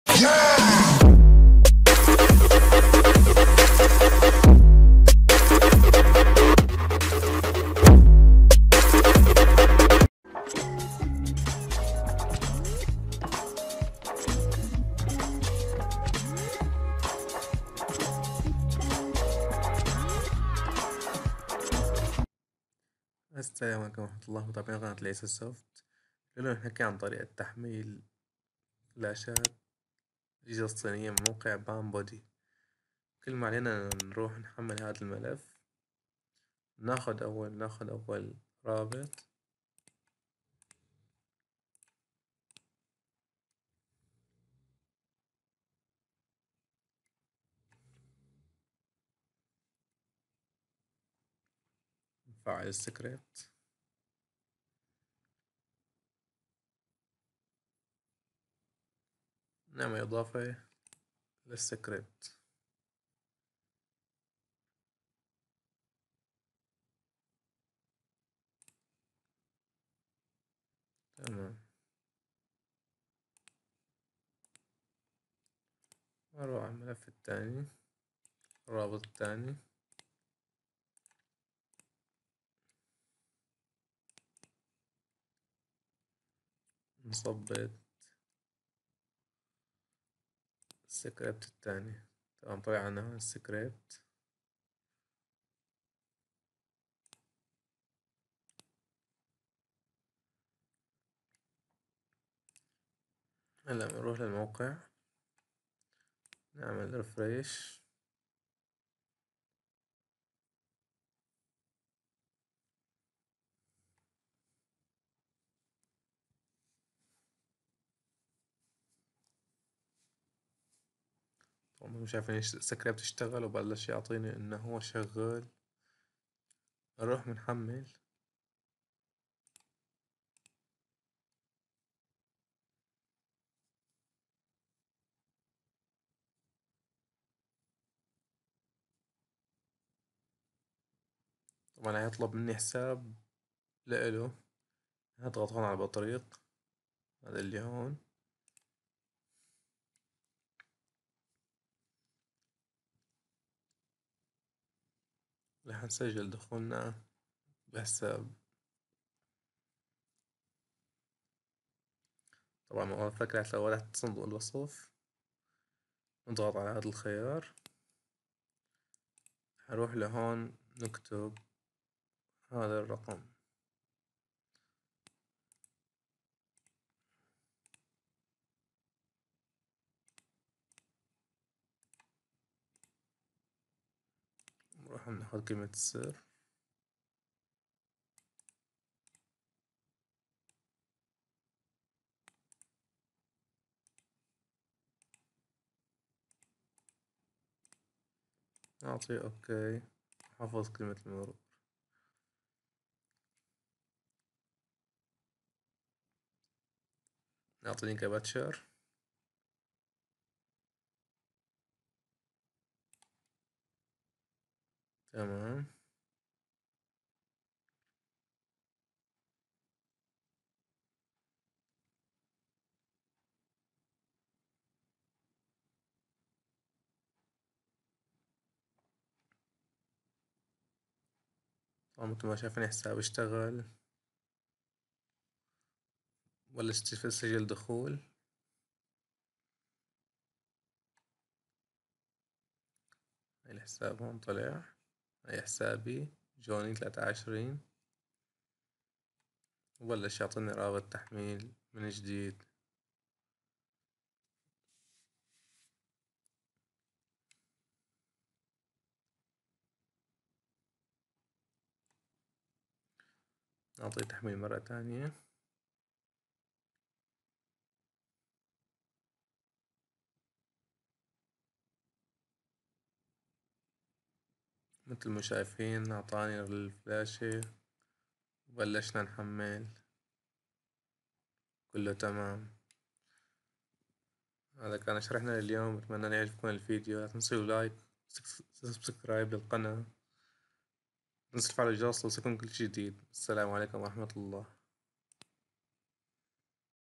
طريقة التحميل من الموقع الصيني PanBuode 2019 %100
السلام عليكم ورحمة االله وبركاته
شرحنا لليوم طريقة تحميل فلاشات من الموقع الشهير
PanBoude
لان اغلب الفنيين عم يعانو من هل المشكلة
بعد سد ثغرة 2017 عن طريق اضافة الكبتشر
رابط تحميل الملف
أخوكم :بلال العيس